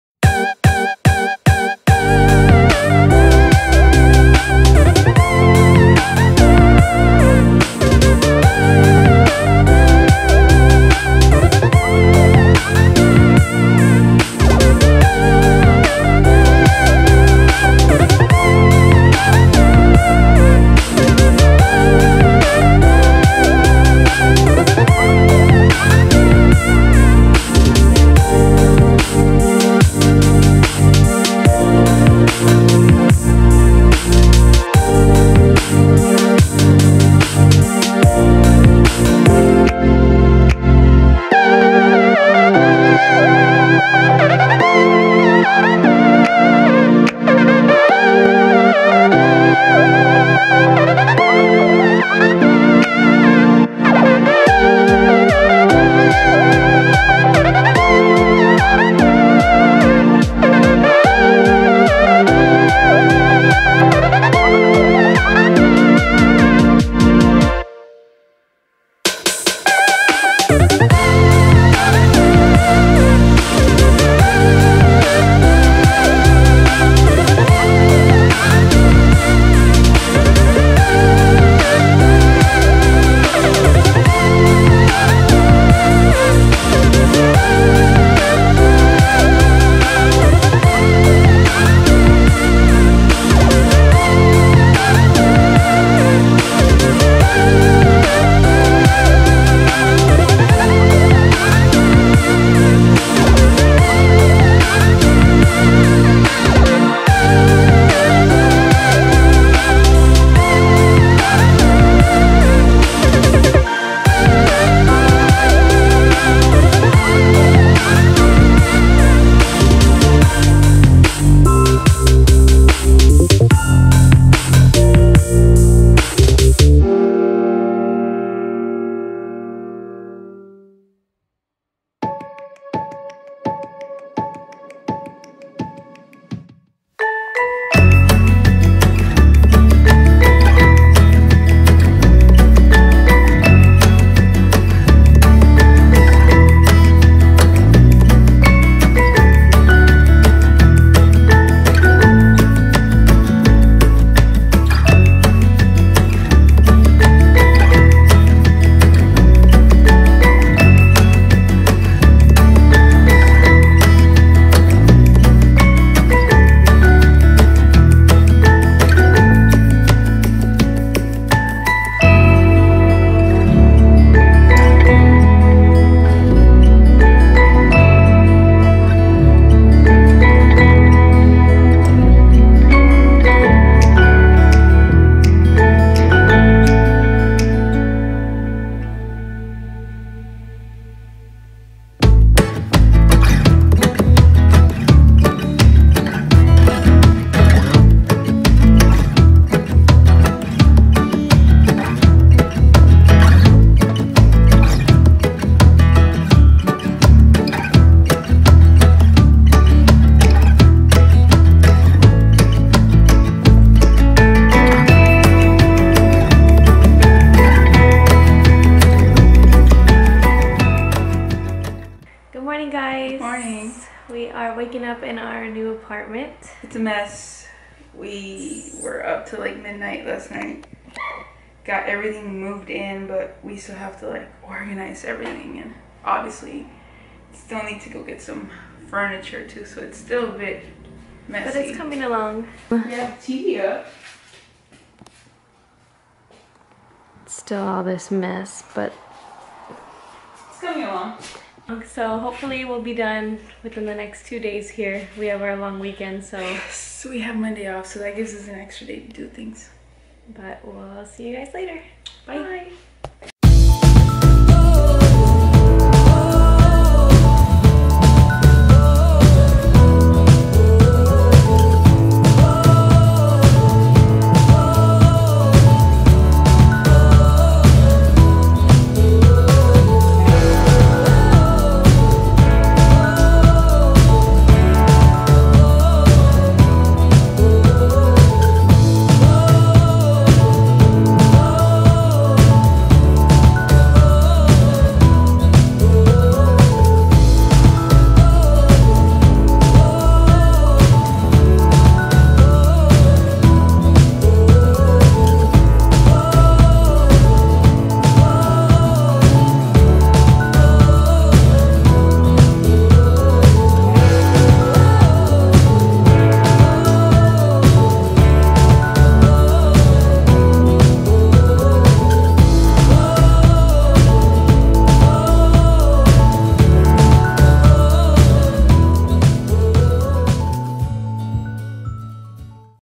Apartment tour part 1!
Enjoy our little apartment tour. Part two is coming next :)

A special thank you to everyone that helped us move!!